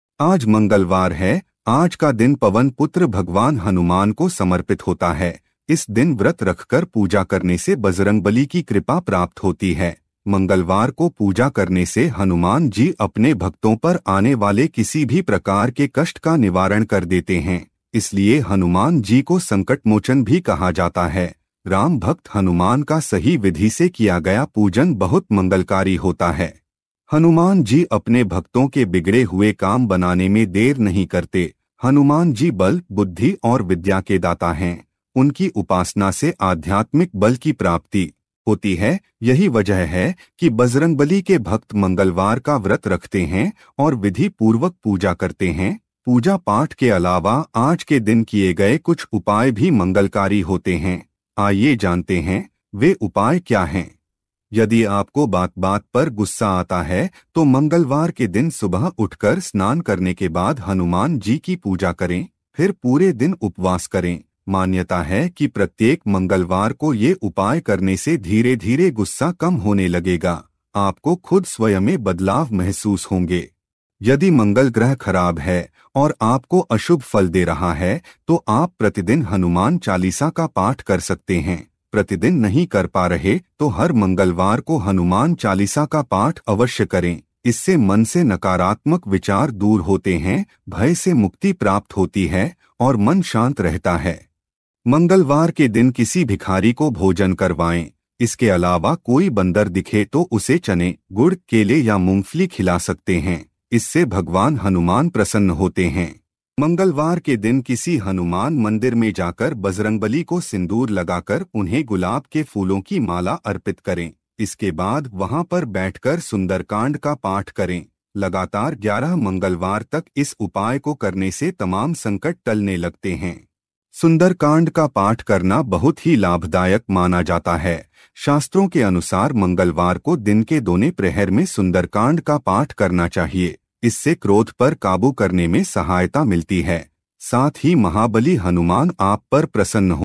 Mangalwar ke Upay मंगलवार को करें बजरंगबली से जुड़े ये उपाय, आर्थिक तंगी होगी दूर, खुलेंगे तरक्की के
Mangalwar ke Upay मंगलवार को करें बजरंगबली से जुड़े ये उपाय, आर्थिक तंगी होगी दूर, खुलेंगे तरक्की के रास्ते